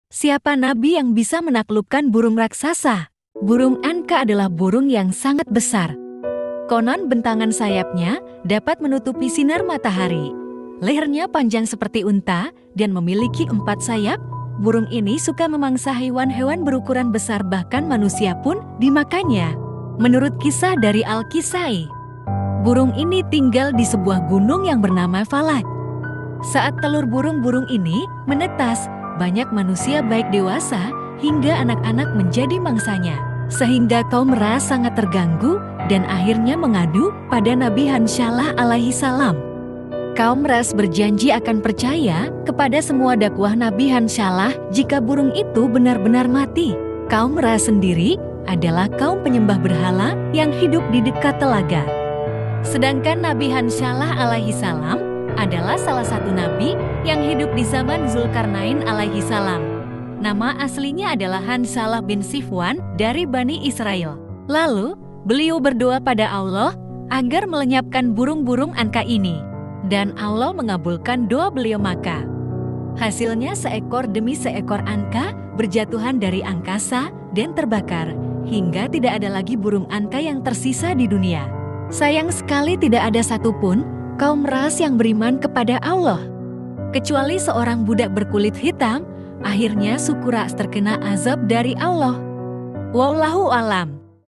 "Siapa Nabi Yang Menaklukkan Burung Raksasa? "
Burung Anqa’ adalah burung yang sangat besar, konon bentangan sayapnya dapat menutupi sinar matahari. Lehernya panjang seperti unta dan memiliki empat sayap. Burung ini suka memangsa hewan-hewan berukuran besar bahkan manusia pun dimakannya.

Menurut kisah dari Al-Kisa'i, burung ini tinggal di sebuah gunung yang bernama Falaj.

Saat telur burung-burung ini menetas, banyak manusia baik dewasa hingga anak-anak menjadi mangsanya.

Sehingga kaum Raas sangat terganggu dan akhirnya mengadu pada nabi Hanzhalah AS.Kaum Raas berjanji akan percaya kepada semua dakwah Nabi Hanzhalah jika burung itu benar-benar mati.

Kaum Raas sendiri adalah kaum penyembah berhala yang hidup di dekat telaga. Sedangkan Nabi Hanzhalah a.s adalah salah satu nabi yang hidup di zaman Dzulkarnain a.s. Nama aslinya adalah Hanzhalah bin Sifwan dari Bani Israil. Lalu Beliau berdoa pada Awlloh agar melenyapkan burung-burung Anqa’ ini.

Dan Awlloh mengabulkan doa beliau maka
Hasilnya seekor demi seekor Anqa’ berjatuhan dari angkasa dan terbakar. Hingga tidak ada lagi burung Anqa’ yang tersisa di dunia.

Sayang sekali tidak ada satu pun kaum Raas yang beriman kepada Allah, kecuali seorang budak berkulit hitam.Akhirnya suku Raas terkena Azab dari Allah SWT.
Wowllahu a, lam.